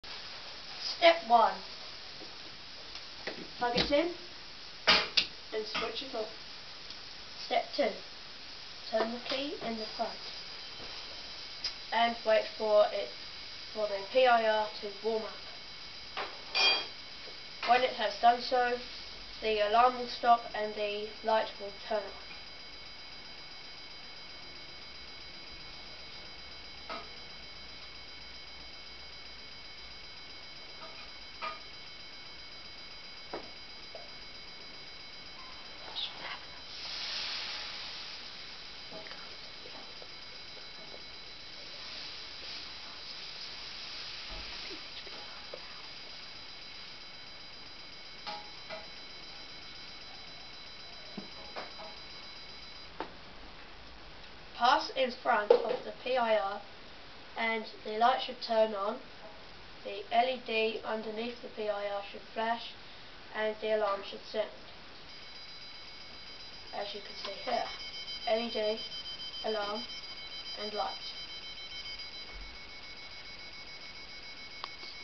Sean Jones - RRG: Robotic Room Guard [Bright Sparks Awards 2009]
Sean needed something to alert him when his brother tried to meddle in his room; robotic room guard provided the answer.